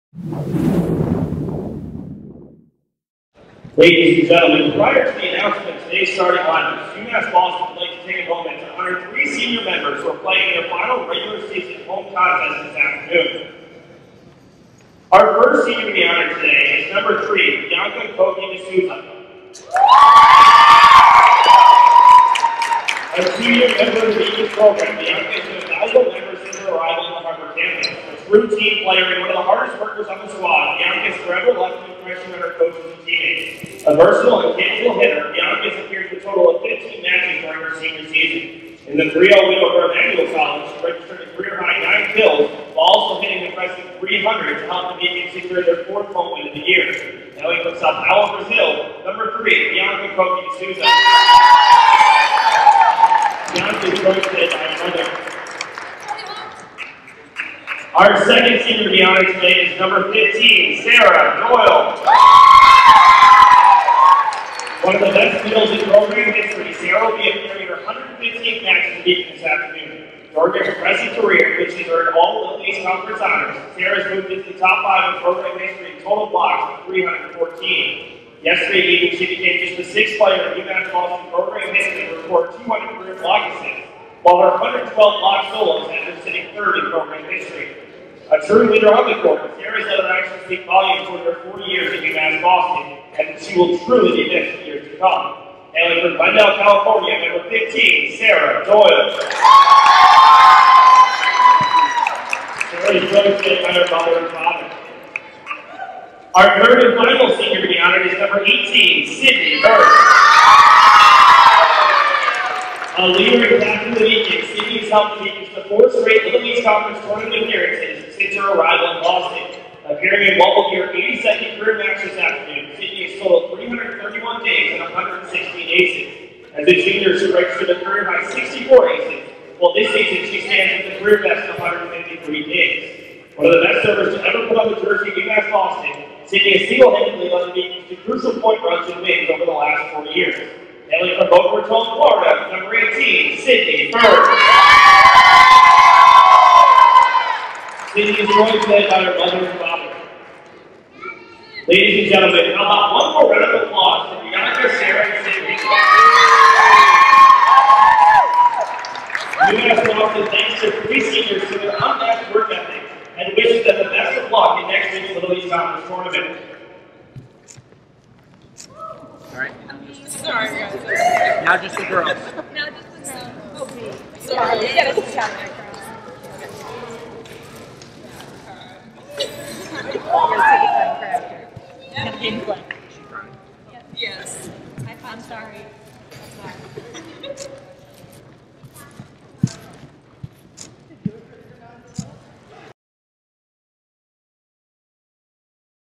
2019 UMass Boston Volleyball Senior Day Ceremony (11/2/19)
UMass Boston Volleyball celebrated their three seniors prior to their final home regular-season contest. The Beacons three seniors helped lead UMass Boston to a 3-2 comeback win over Brandeis University.